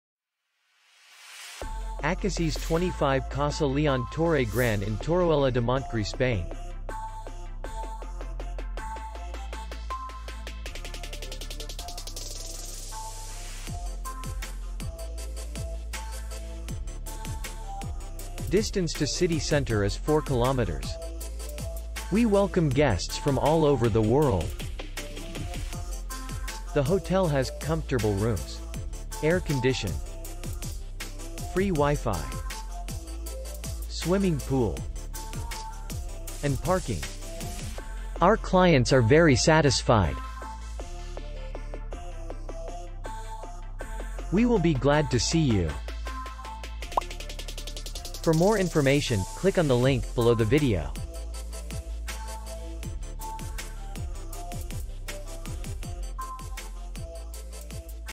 Acacies 25 Casa Leon Torre Gran hotel review | Hotels in Torroella de Montgri | Spain Hotels
Acacies 25 Casa Leon Torre Gran hotel video review. Hotels in Torroella de Montgri, european 3 stars hotels.